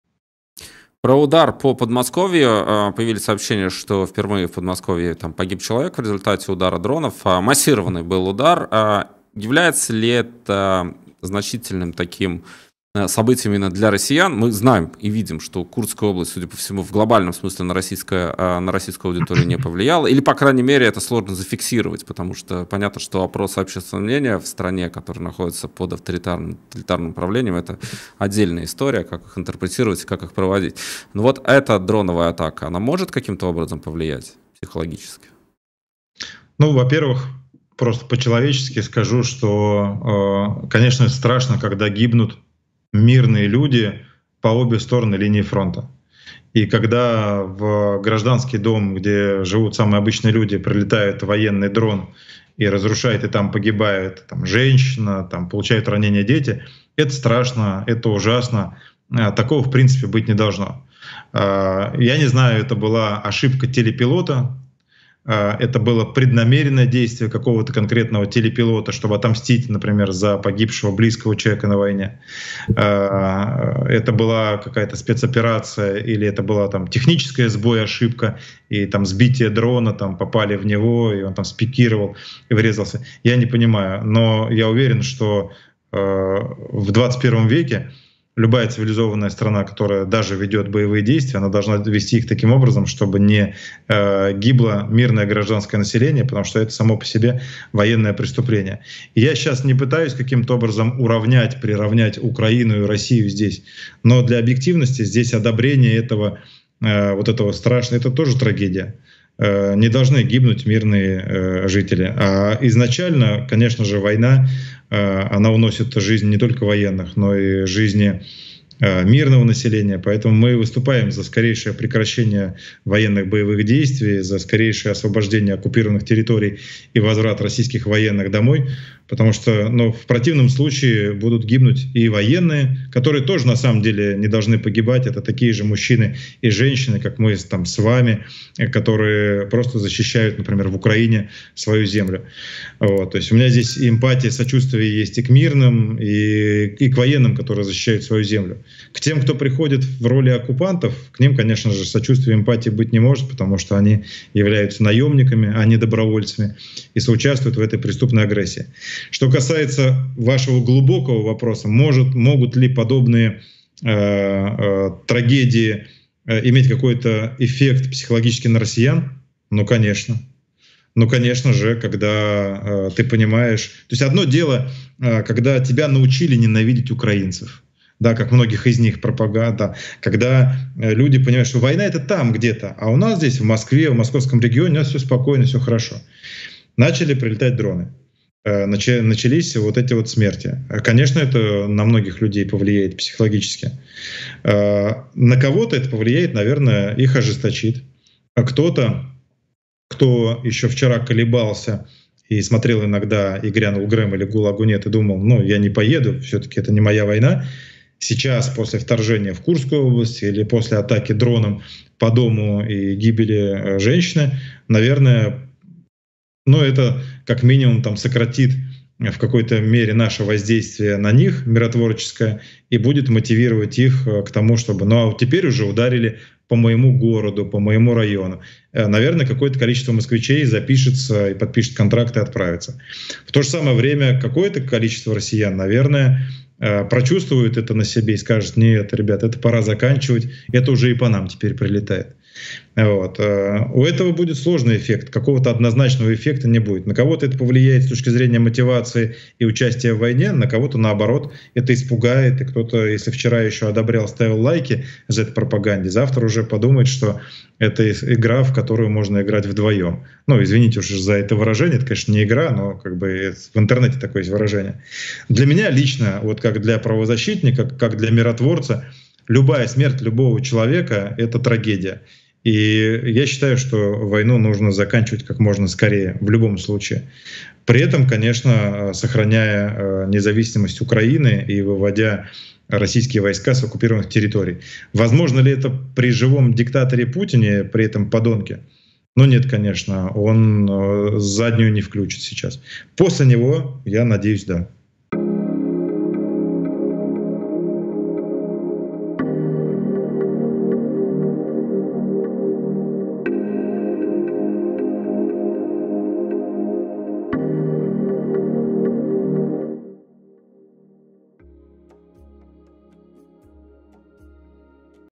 ВЗРЫВЫ в Москве! Пылают многоэтажки. Массированный налет дронов. Шокирующая реакция русских
RU - Массированная атака дронов на Подмосковье. Как отреагировали россияне и каким может быть эффект? Детальнее об этом рассказал Владимир Осечкин в эфире @i_gryanul_grem

UA - Масована атака дронів на Підмосков'ї. Як відреагували росіяни та яким може бути ефект? Детальніше про це розповів Володимир Осєчкін в ефірі @i_gryanul_grem